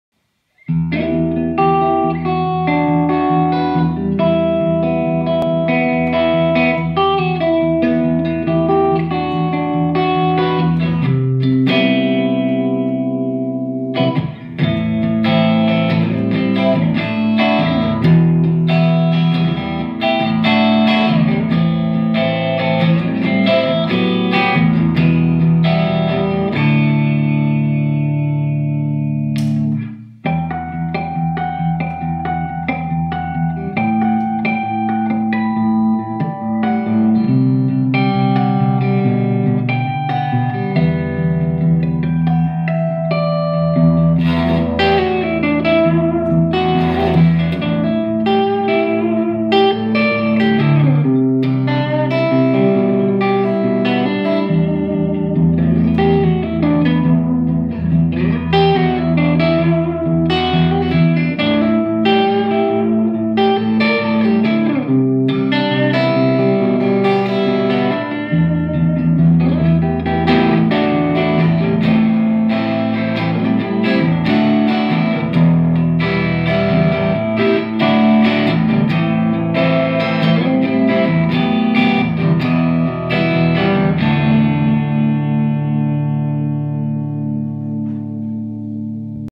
Red Bird Instruments - FFastback
New RedBird’s model, FFastback, played by luthier Jan Fic in RedBird’s showroom.